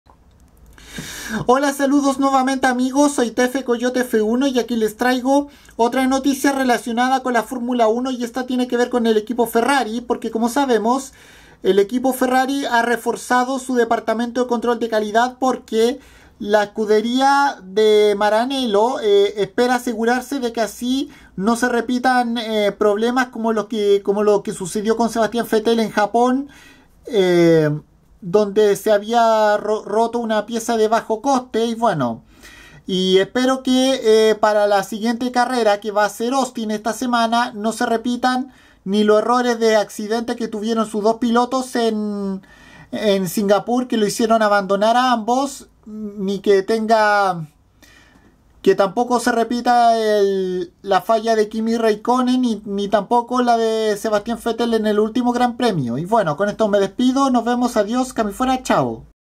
El equipo Ferrari refuerza su departamento de control de calidad
El equipo de Maranello se asegura de que no se vuelva a repetir fallos como el del Gp de Japón donde una pieza de menos costo salió averiada